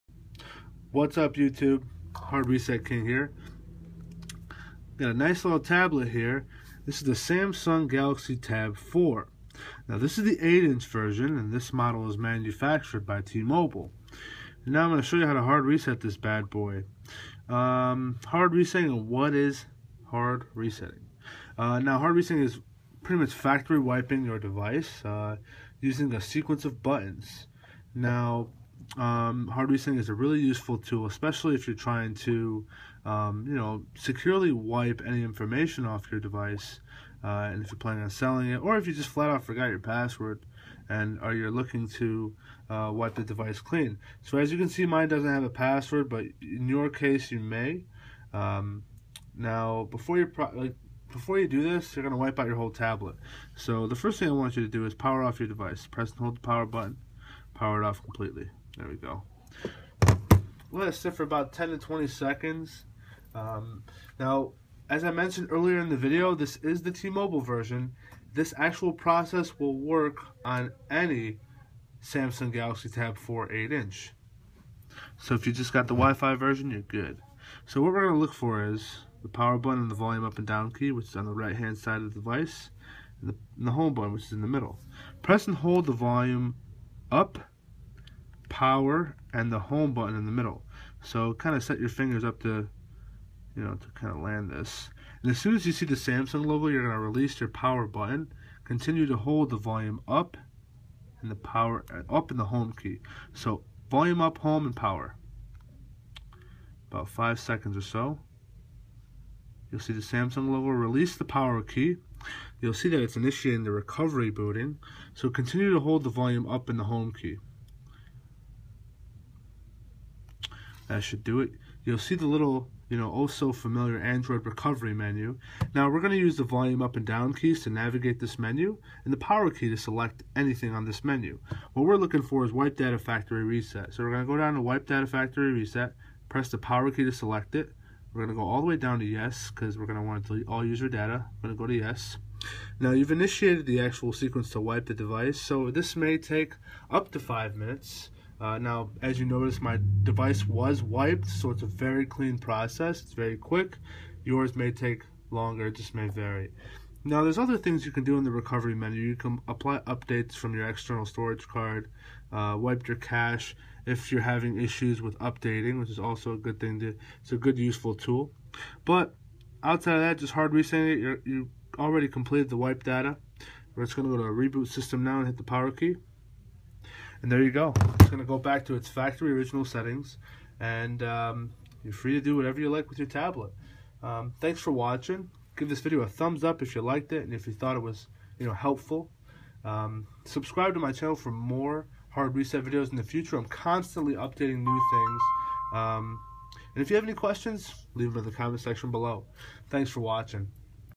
How to hard reset The Samsung Galaxy Tab 4 8.0 Android 5.0 Lollipop Remove Password
Android Recovery Menu

Android 5.0 Lollipop

Remove Password, Hard Reset

Volume Up, Home and Power

Follow Recovery Menu